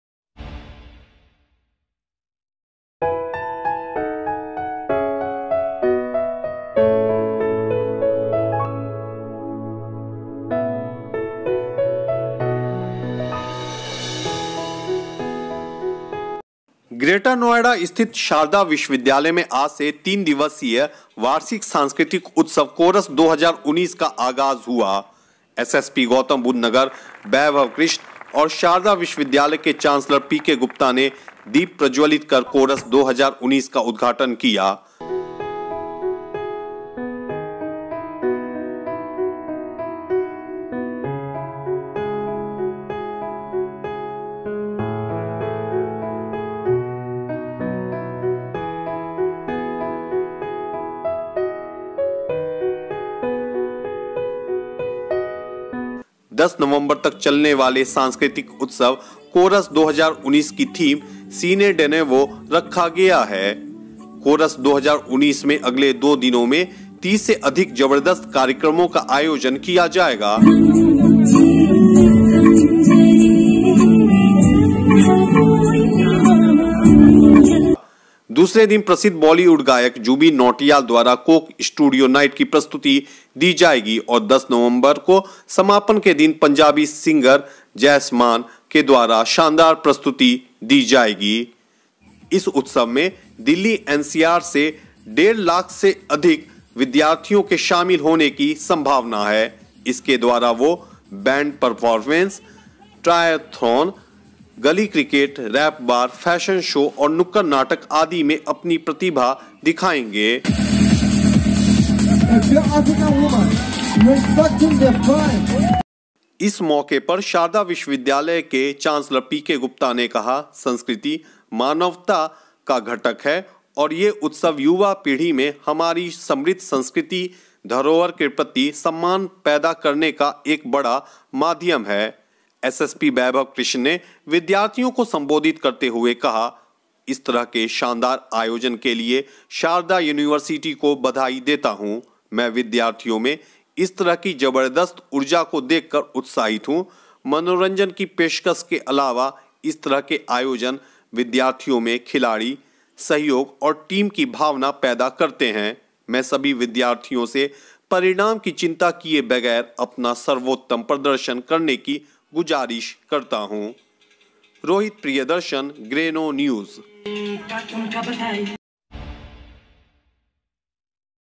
Chorus 2019 at Sharda University l l Grenonews ll शारदा विश्विद्यालय में कोरस 2019
- शारदा युनिवर्सिटी ने वार्षिक सांस्कृतिक उत्सव कोरस 2019 का उद्घाटन किया

- किसी संगीत, नृत्य, कला, नाटक और मनोरंजन का एक आदर्श मेल, यह उत्सव तीन दिन चलेगा

दिल्ली एनसीआर, 8 नवंबर, 2019ः दिल्ली एनसीआर में ग्रेटर नोएडा स्थित अग्रणी शैक्षणिक संस्थान शारदा युनिवर्सिटी ने इस साल का अपना सबसे बड़ा उत्सव "कोरस 201" 8 नवंबर, 2019 को शुरू किया। इस साल के उत्सव की थीम रखी गई है cinedenovo जो नवंबर के महीने में सिनेमाई विचार के साथ गूंजता है। तीन दिनों तक चलने वाले इस उत्सव में दिल्ली एनसीआर से 1.5 लाख से अधिक विद्यार्थियों के शामिल होने की संभावना है।
कोरस का 10वां संस्करण मुख्य अतिथि और गौतम बुद्ध नगर के वरिष्ठ पुलिस अधीक्षक श्री वैभव कृष्ण द्वारा दीप प्रज्ज्वलित कर शुरू किया गया। इसके बाद शारदा युनिवर्सिटी के कुलपति डाॅक्टर जीआरसी रेड्डी ने संबोधित किया। स्टूडेंट वेलफेयर की डीन डाॅक्टर निरूपमा गुप्ता ने अतिथियों का स्वागत किया। प्रख्यात कथक नृत्यांगना, पद्मश्री और एसएनए पुरस्कार विजेता डाॅक्टर शोवना नारायण ने उद्घाटन समारोह में अपनी विशेष प्रस्तुति से लोगों को मंत्रमुग्ध कर दिया।
अपने उद्घाटन भाषण के तहत विद्यार्थियों को संबोधित करते हुए गौतम बुद्ध नगर के एसएसपी श्री वैभव कृष्ण ने कहा, श्मैं इस तरह के शानदार आयोजन के लिए शारदा युनिवर्सिटी को बधाई देता हूं। मैं विद्यार्थियों में इस तरह की जबरदस्त ऊर्जा को देखकर उत्साहित हूं। मनोरंजन की पेशकश के अलावा इस तरह के आयोजन विद्यार्थियों में खिलाड़ी, सहयोग और टीम की भावना पैदा करते हैं। मैं सभी विद्यार्थियों से परिणाम की चिंता किए बगैर अपना सर्वोत्तम प्रदर्शन करने की गुजारिश करता हूं।

इस अवसर पर शारदा युनिवर्सिटी के कुलाधिपति श्री पीके गुप्ता ने कहा, श्हमें कोरस का 10वां संस्करण आयोजित करने का सौभाग्य मिला है जिसके तहत विभिन्न युनिवर्सिटीज के विद्यार्थियों के पास एक मंच पर आकर अध्ययन से परे नेटवर्क बनाने का मौका है। इस तरह के उत्सव ऐसे मंच हैं जहां युवा एक दूसरे से संवाद करते हैं, अपने आसपास की ताजा घटनाओं को साझा करते हैं और अपनी प्रतिभा प्रदर्शित करते हैं। काॅलेज के सांस्कृतिक उत्सव इस पाठ्यक्रम का एक महत्वपूर्ण हिस्सा हैं l